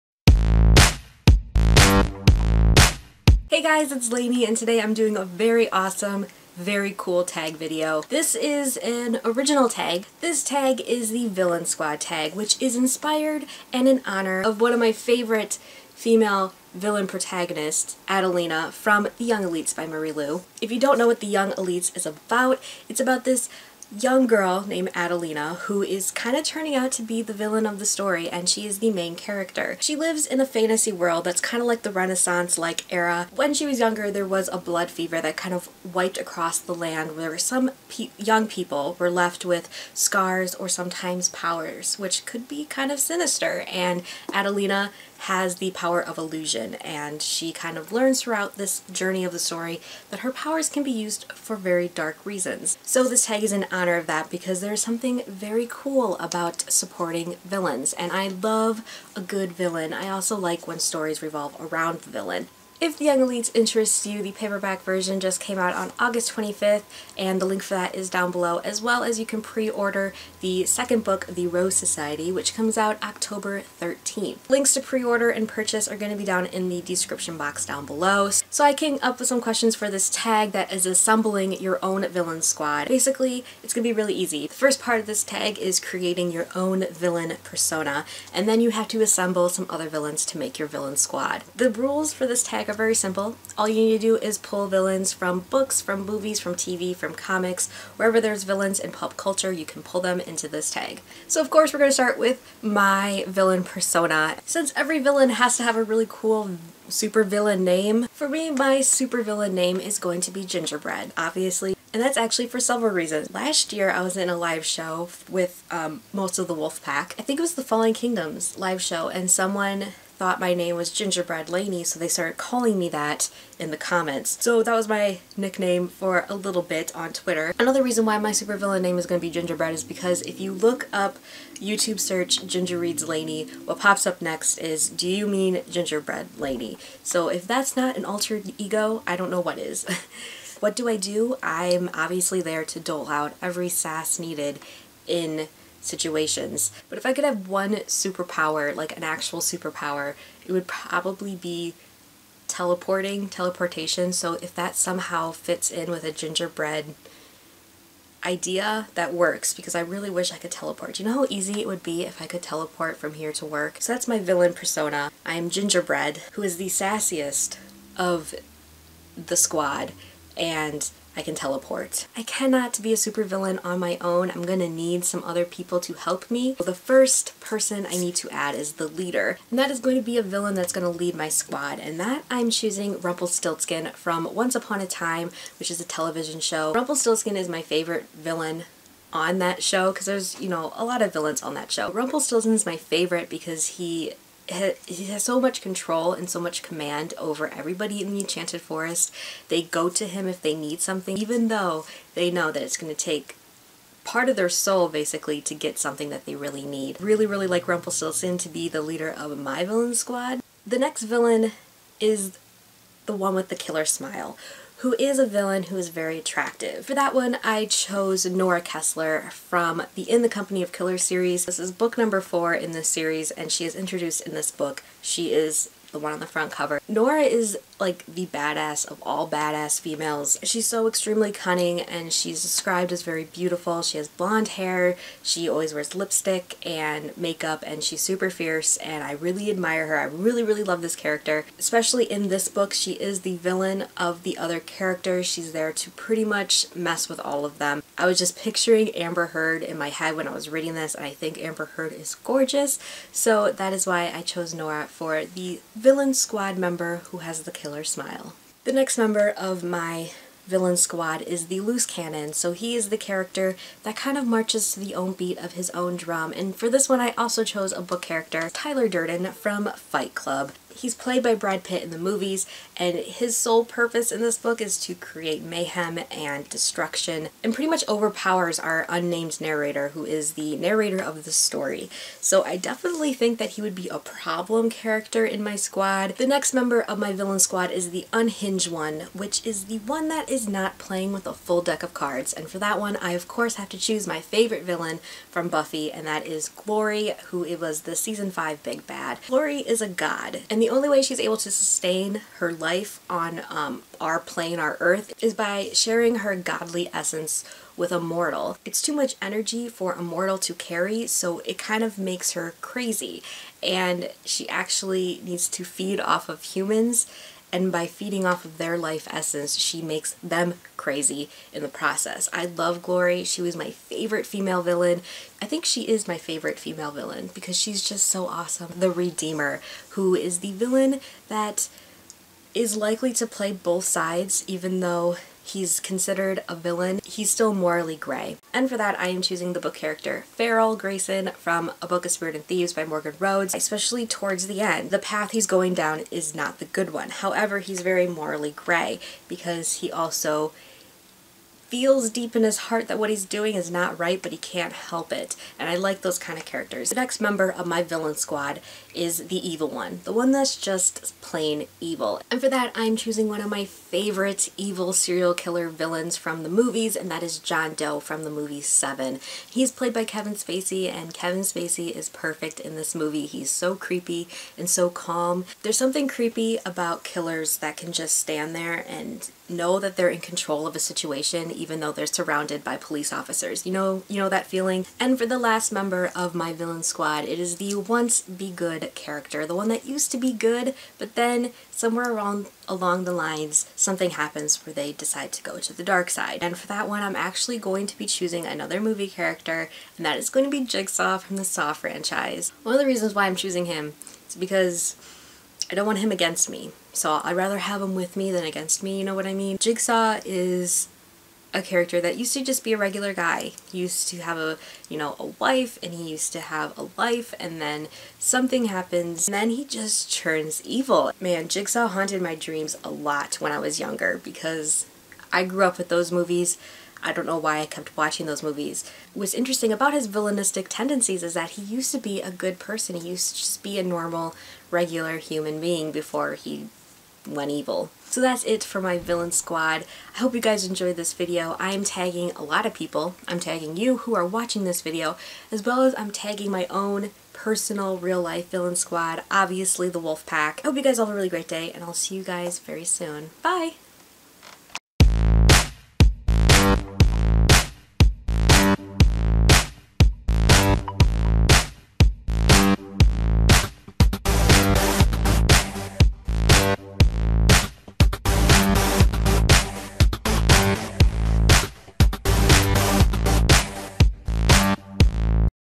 VILLAINSQUAD TAG!
If you could assemble your own Villain Squad, who would be in it? This tag was inspired by THE YOUNG ELITES by Marie Lu. Thank you Penguin Teen!

Kassidy's video:
Mallory's video:

THE QUESTIONS:
- Create your own villain persona (name, skills, etc)
1. The Leader - Pick a villain to lead the squad.
2. The Killer Smile - Pick a villain who's just extremely attractive
3. The Loose Canon - Pick a villain that marches to the beat of their own drum
4. The Unhinged One - Pick a villain that isn't playing with a full deck.
5. The Redeemer - Pick a villain that's on a path of redemption/would play both sides
6. The Evil One - Pick a villain that is just bad to the bone.
7. The Once-Was-Good - Pick a villain that didn't originally start out as a villain.